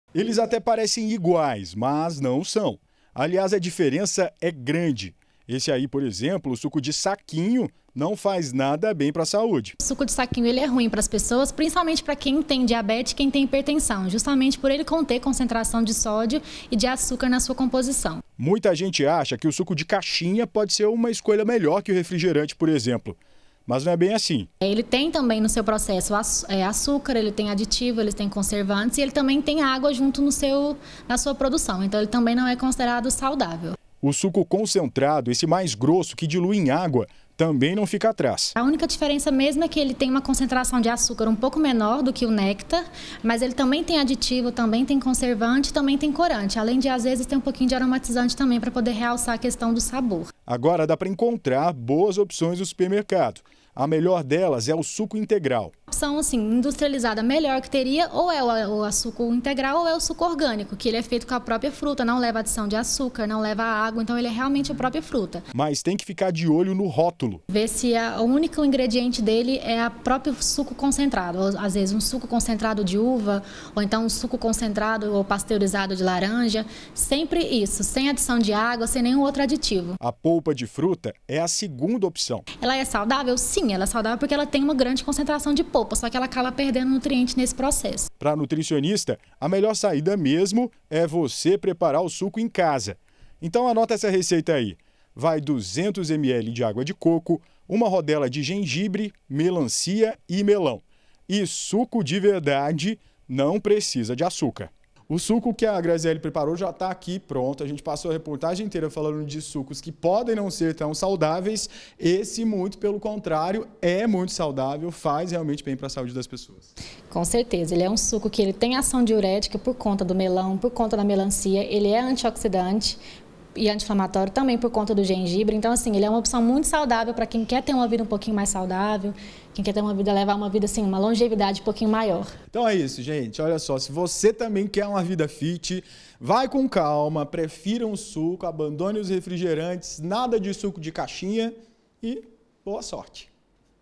RICOS EM AÇÚCAR: SUCOS INDUSTRIALIZADOS FAZEM MAL A SAÚDE
Tem gente que acredita que suco é mais saudável que refrigerante. Mas o que muita gente não sabe é que nem todo suco faz bem a saúde. Os sucos industrializados, aqueles de saquinho e de caixinha, têm grande quantidade de açúcar, conservante e aromatizante. Tudo isso faz um mal danado pra saúde.